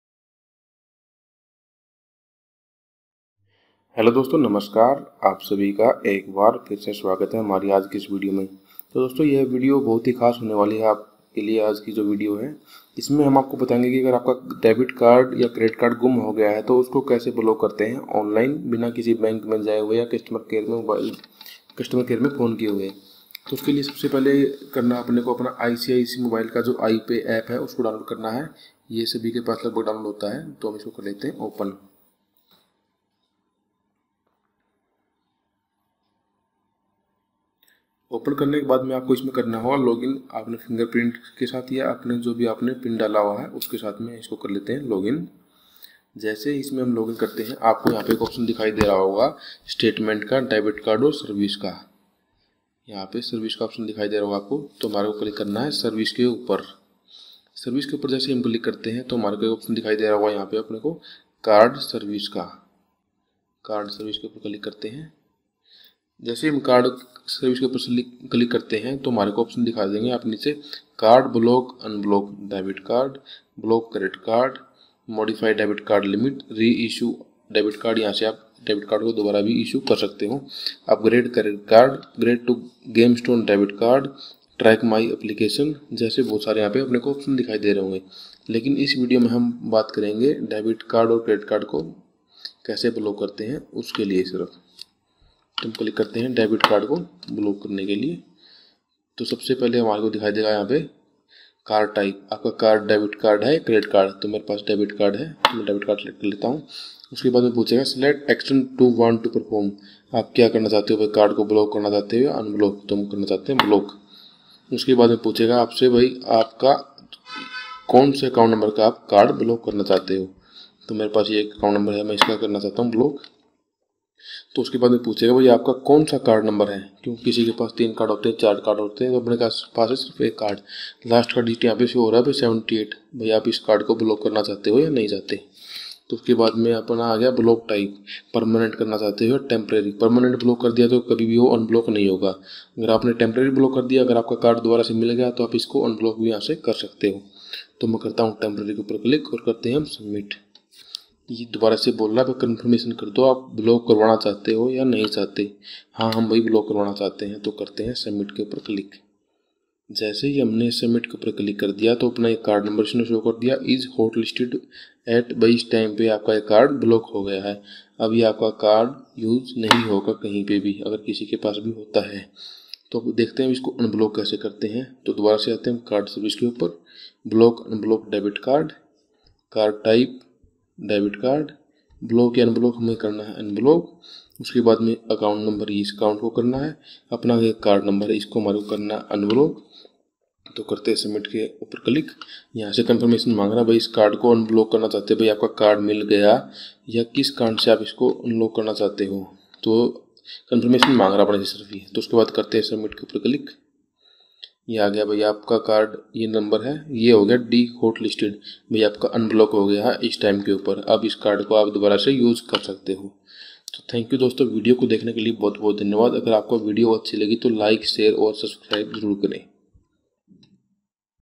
How to block / unblock icici bank atm card online | icici bank atm card ko block kaise kare online
How to block / unblock icici bank debit card online | icici debit card ko block kaise kare online

My Laptop

My Mike

My Dslr